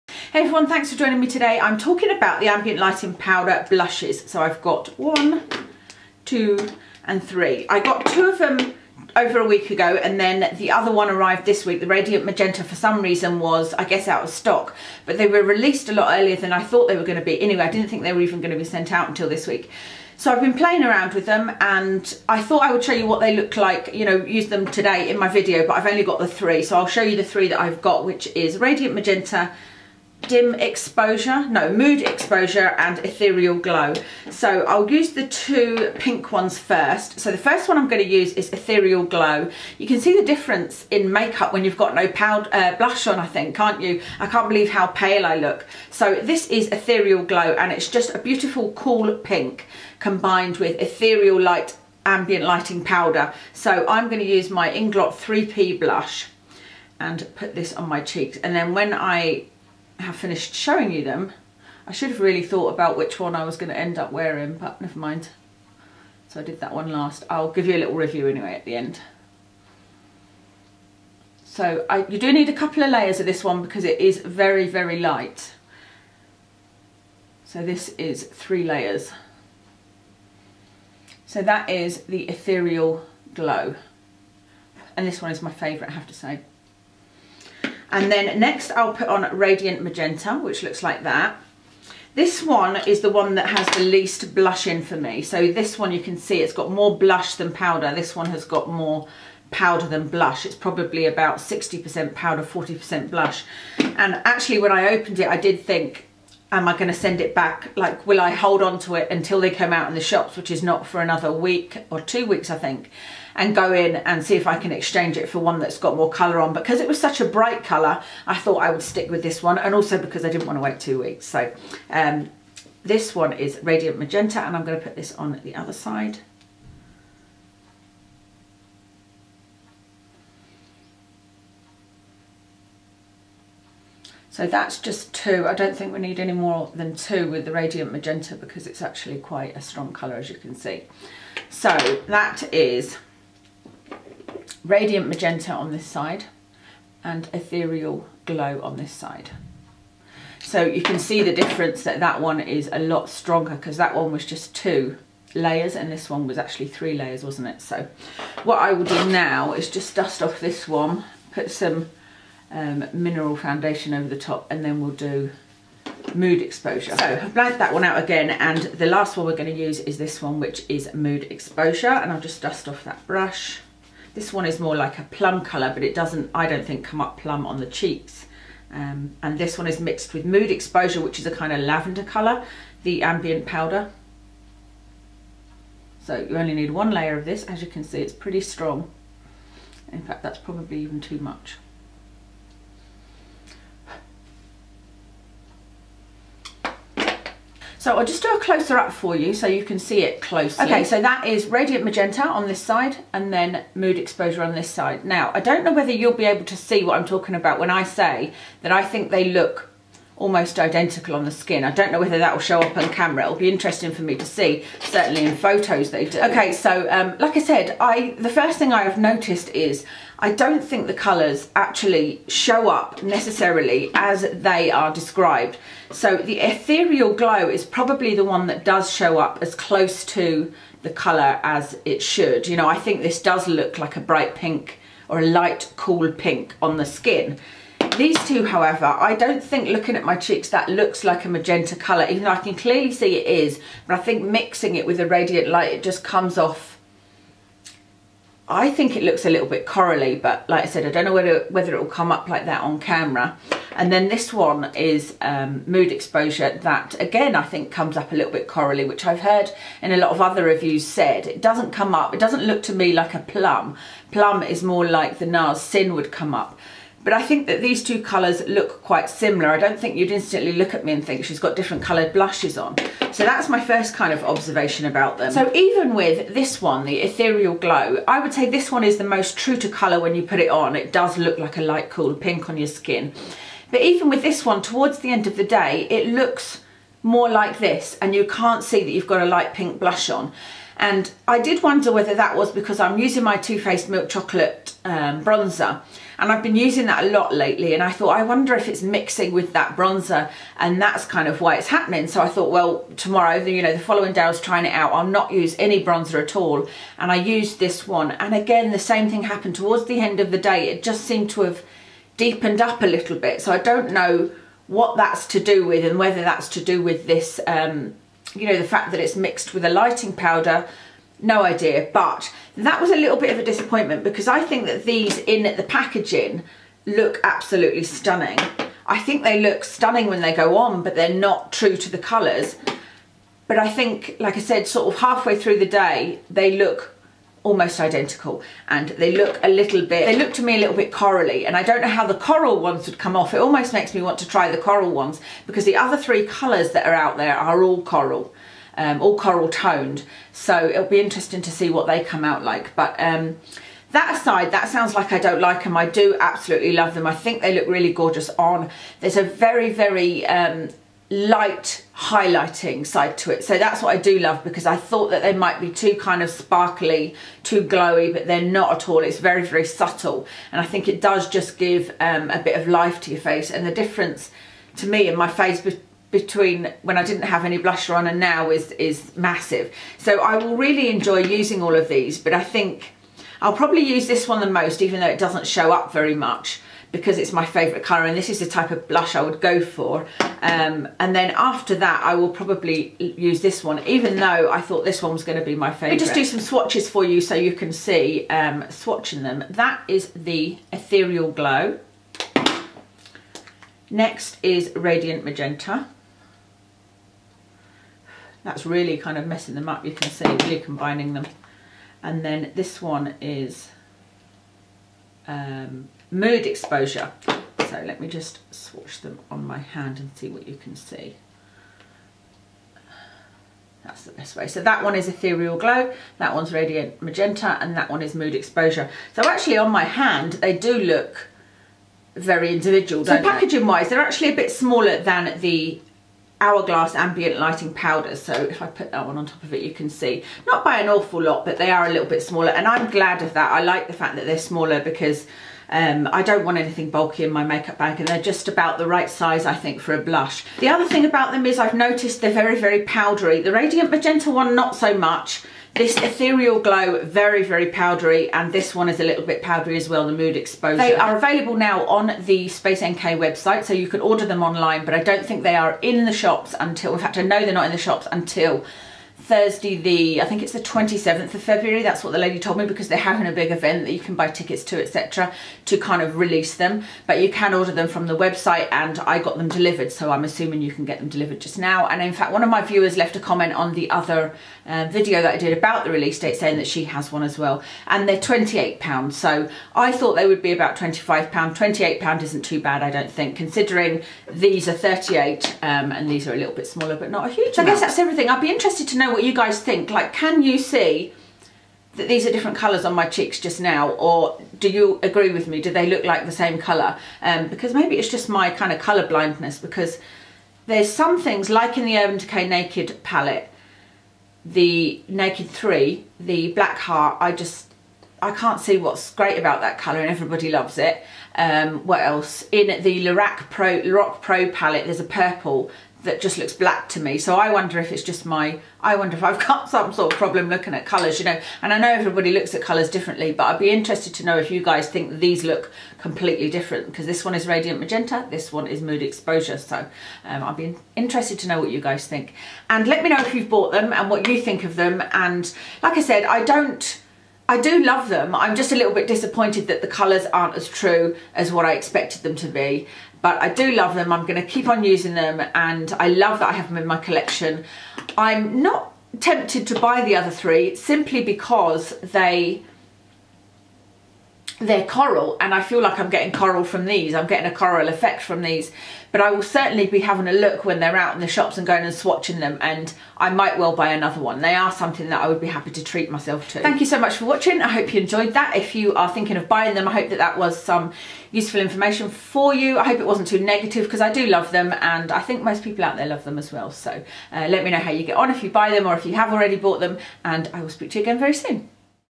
Hourglass Ambient Lighting Blush Review - Radiant Magenta, Ethereal Glow, Mood Exposure
A review of the 3 ambient lighting blushes that I bought. are they worth the hype & money?!

Makeup & Nails today:
No7 Stay perfect super light foundation in Calico
Bourjois healthy mix under eye concealer in 51light radiance
Bourjois healthy balance face powder in 52 Vanilla
Hourglass ambient lighting blush in Radiant Magenta and Mood Exposure
MAC paint in Untitled
Urband Decay naked 3 palette - Trick, Limit, Factory
Makeup Geek gel eyeliner
MAC extended play gigablack mascara
Maybelline superstay lipstick in 615 taupe
MAC brow pencil in lingering
Nails - Maybelline colour show nails in black, & Collection green nail polish